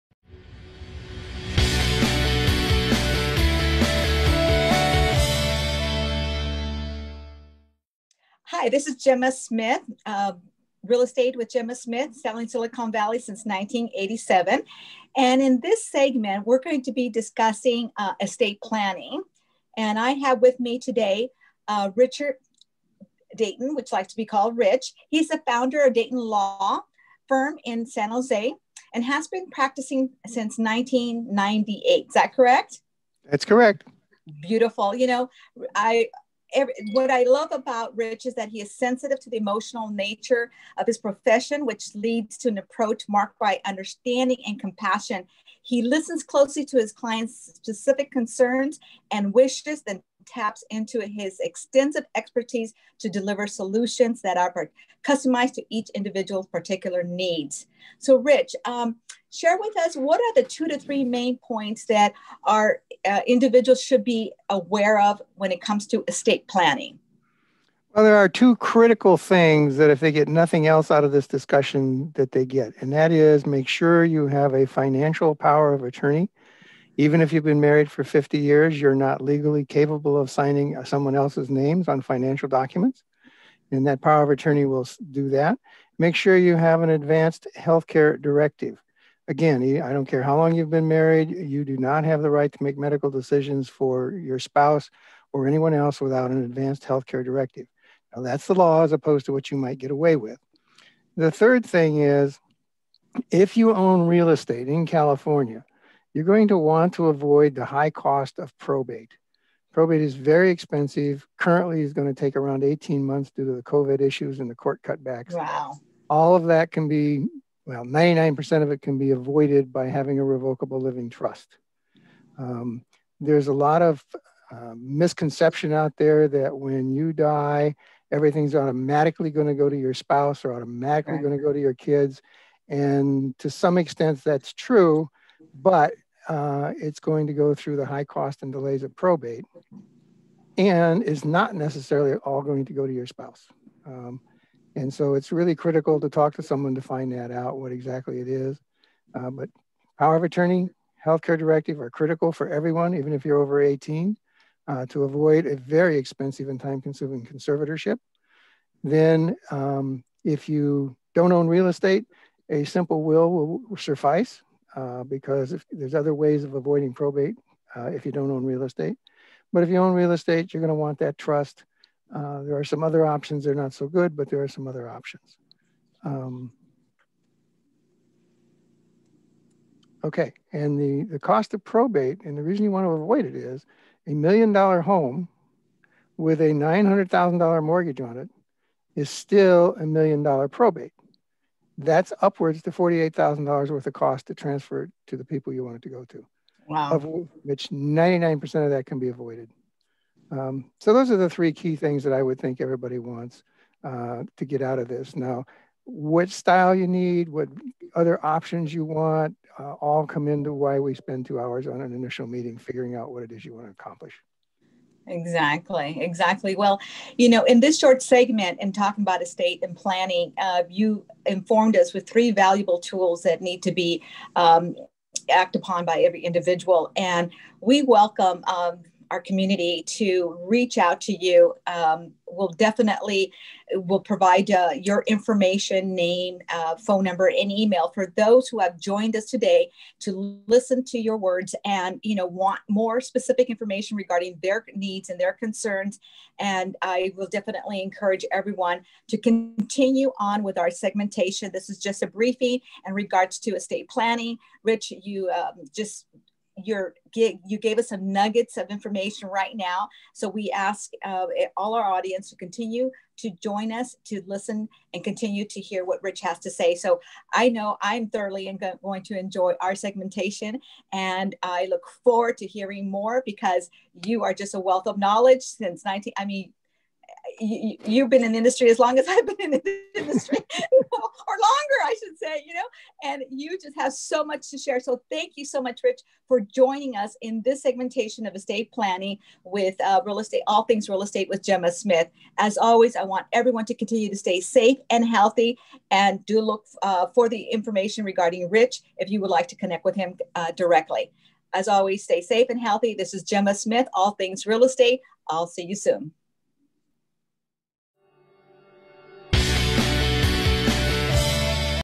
All Things Real Estate with Gema Smith - Episode 1: Estate Planning with Attorney Rich Dayton
Welcome to All Things Real Estate with Gema Smith.

In this segment we're going to be discussing estate planning with Richard Dayton.

He's the founder of Dayton law firm in San Jose, CA and has been practicing since 1998

Contact Gema Smith
DRE #: 01128805

Richard C. Dayton
Certified Specialist in Estate Planning, Probate and Trust Law
By the State Bar of California. Board of Specialization
The Dayton Law Firm